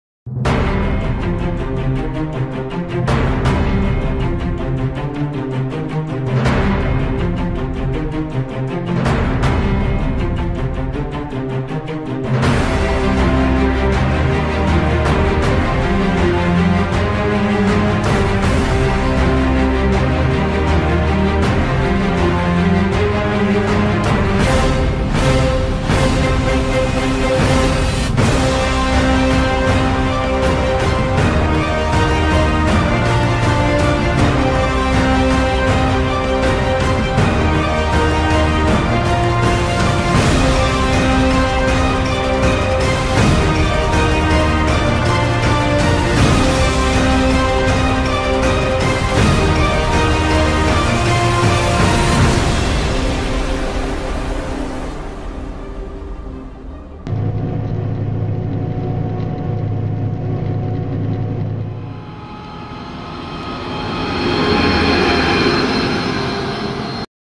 Již brzy / Coming Soon | Třemošenská a.s. | Claas,Fendt,JCB,John Deere and others
CZ/SK:     Malá ukázka z toho, na co se můžete v následujících dnech a týdnech těšit !!  Celkem na 12 nových videí !!  :)
ENG:     A small sample of it, what you can in the coming days and weeks to enjoy !!  A total of 12 new videos !! :)

Fotoaparát/ Camera:    Panasonic Lumix DMC-FS10, HTC Desire 610

Doufám, že se bude líbit a podpoříte mě !  :)
I hope you will enjoy and will support me! :)